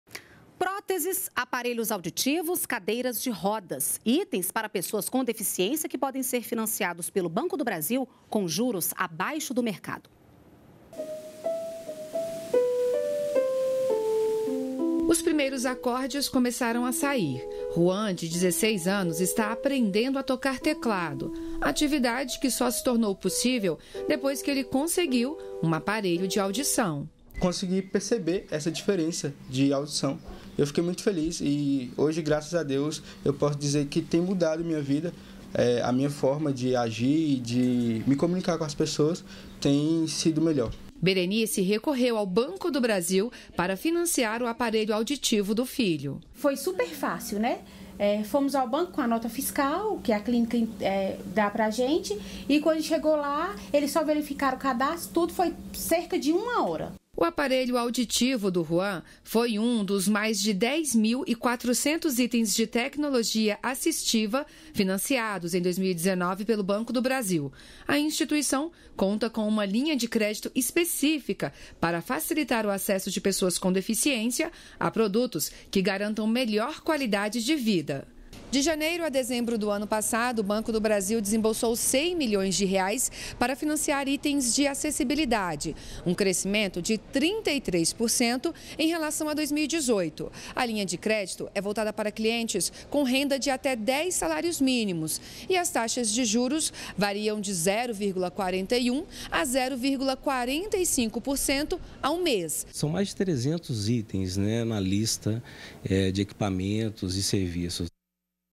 Linha de Crédito Especial para Itens de Acessibilidade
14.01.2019 - Próteses, aparelhos auditivos e cadeiras de rodas são alguns dos itens para pessoas com deficiência que podem ser financiados pelo Banco do Brasil com juros abaixo do mercado.